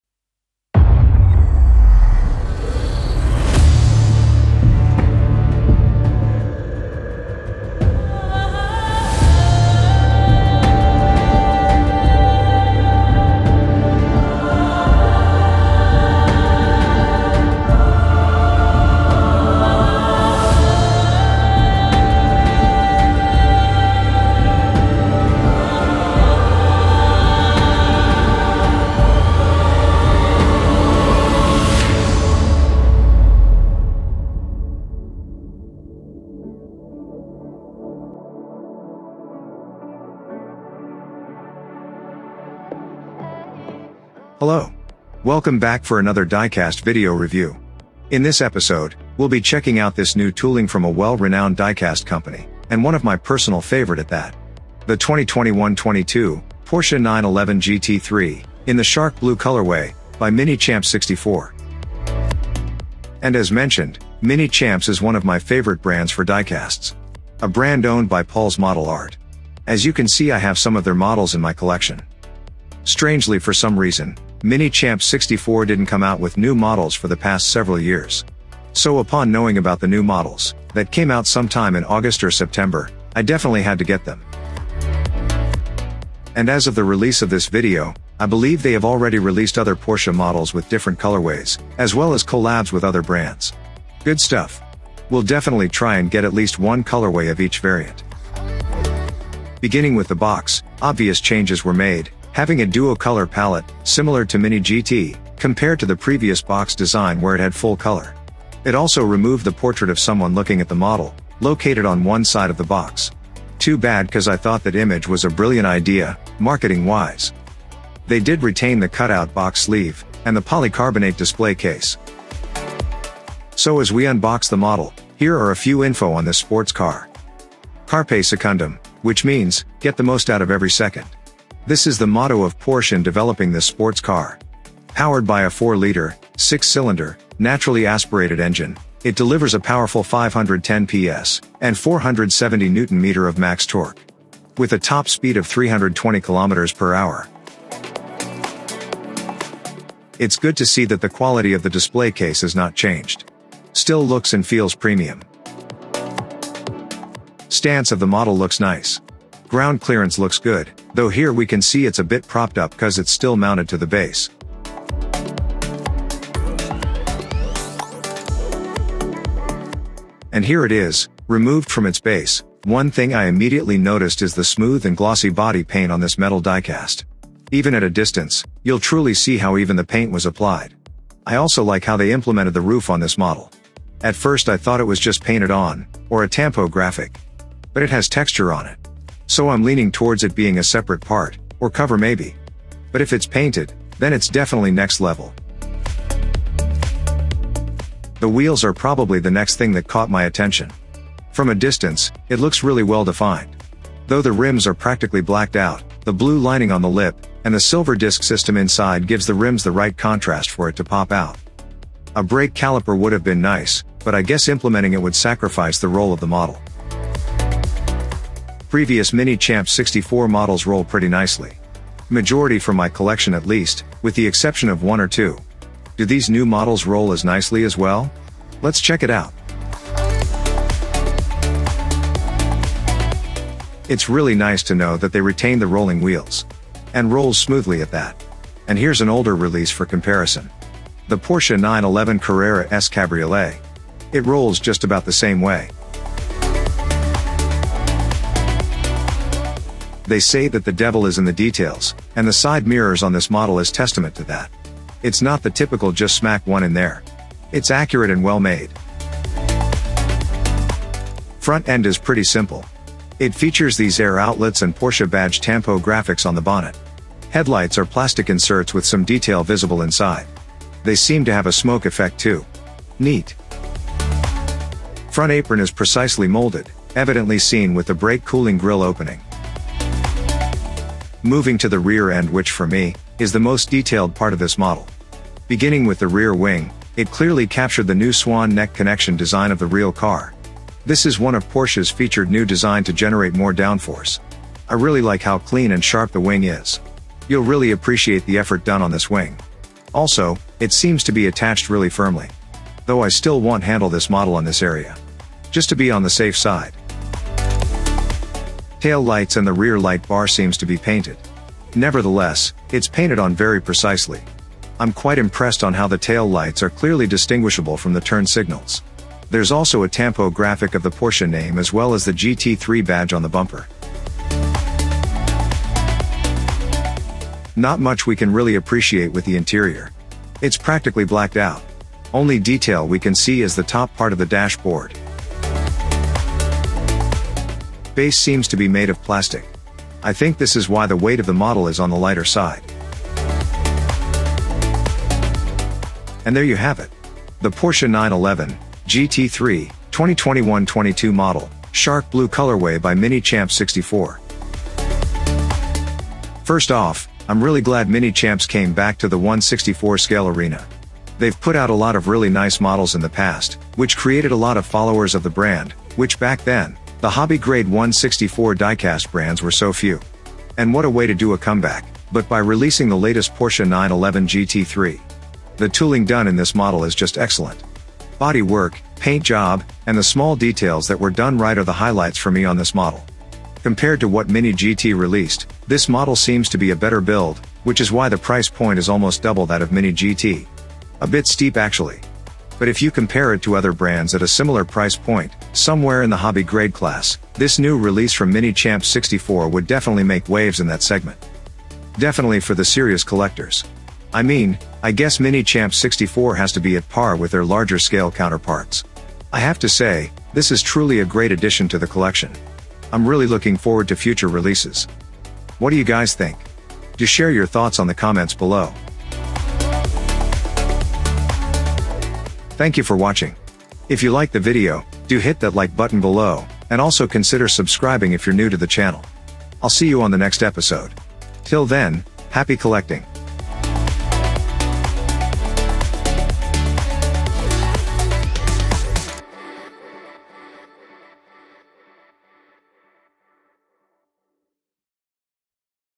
MINICHAMPS 64 is back! Porsche 911(992) GT3, Shark Blue. Unboxing and Review!
Hello fellow 1/64 diecast collectors. Welcome to Studio 64. Tis the time to celebrate the return of Minichamps 64! After a long absence fro the 64 diecast scene, they're back. And back with a bang at that. Check out this new Porsche 911 GT3.

Video Clip:  The New 911 GT3 - "Time is Precious"

remixed by Studio64 to fit video length